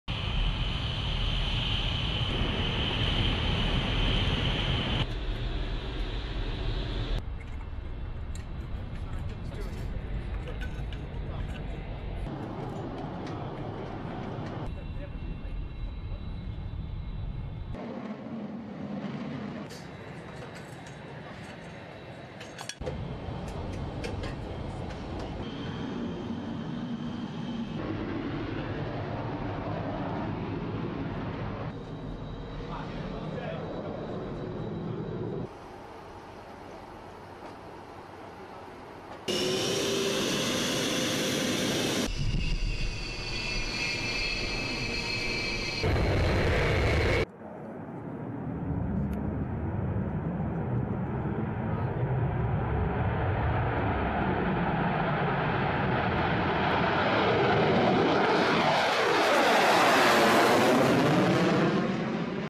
Russia Su-34 fighter bombers 'equipped with air-to-air missiles', Syria
Russian Su-34 fighter bombers flew in Syria for the first time with air-to-air missiles for self-defence on Monday (November 30), according to the Russian Defence Ministry.

This comes less than a week after a Turkish F-16 shot down a Russian jet leading to a nosedive in Russia-Turkish relations.

The air-to-air missiles are capable of hitting targets at a distance of up to 60 kilometres (37.28 miles), the ministry said. It added the use of short and medium range missiles were necessary to "provide security" for Russian air forces.

Russian President Vladimir Putin said on Monday the reason Turkey downed the Russian warplane last week was that it wanted to protect supplies of oil from Islamic State.